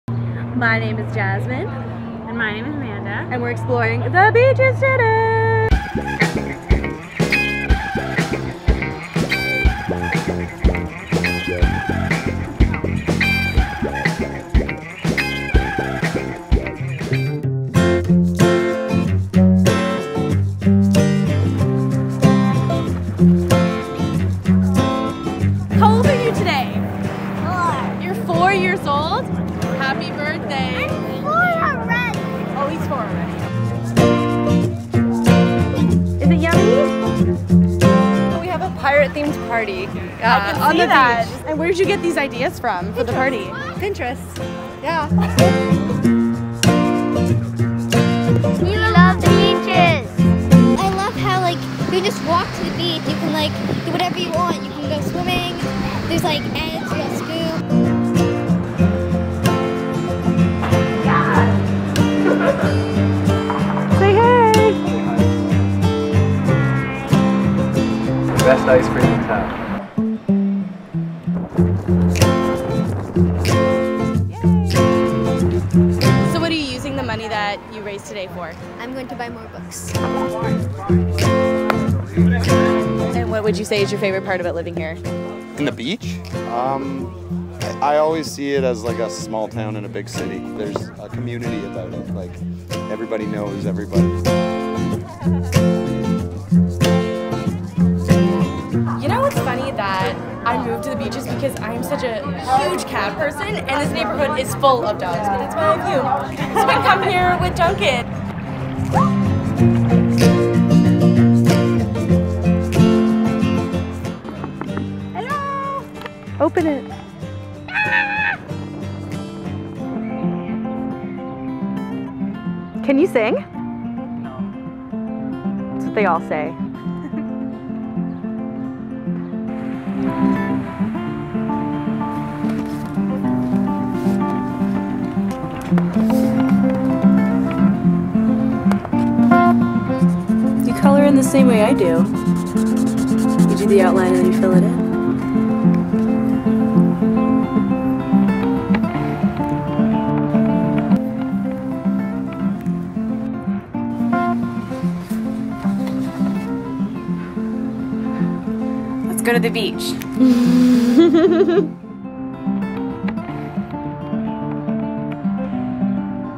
A Day at The Beach(es) | Girl About Toronto TV
In this episode, Amanda Cosco and Jasmine Pazzano spend a Saturday strolling through Toronto's Beach neighbourhood, which is widely known as The Beaches. The area includes Kingston Road Village and the easternmost stretch of Queen Street East.